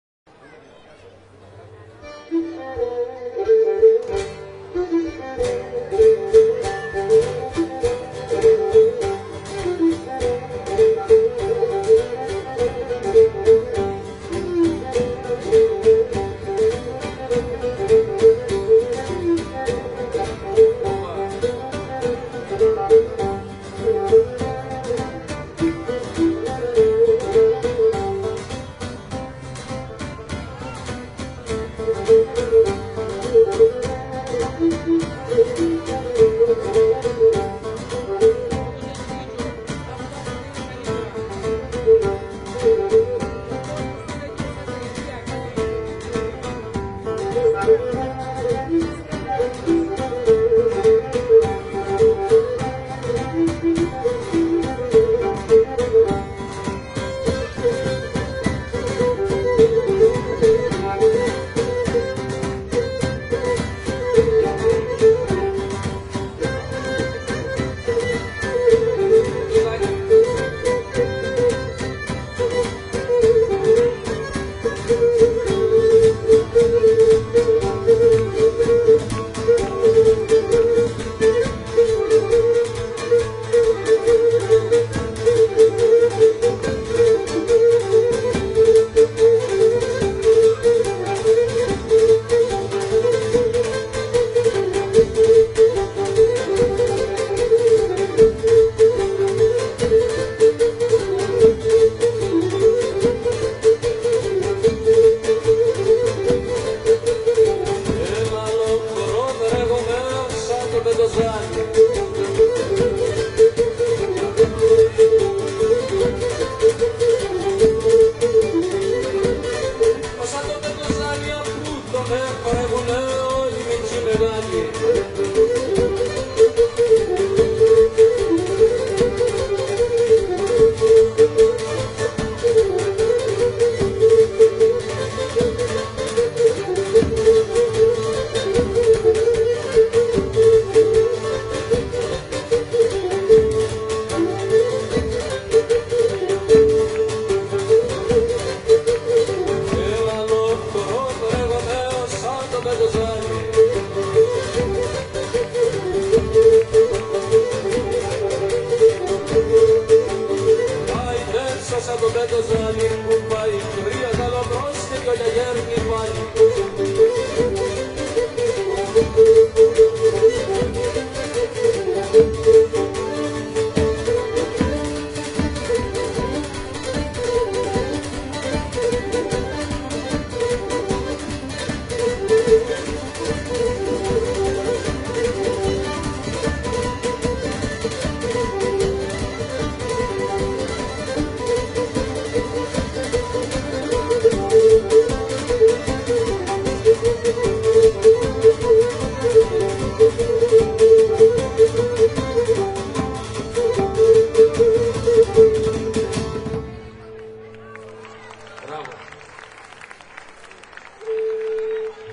Δημήτρης Σγουρός-Σιγανός Πεντοζάλης, Μοχός 2010
Λήψη από το πανηγύρι της Παναγίας τον Αύγουστο του 2010 στην πλατεία του Μοχού.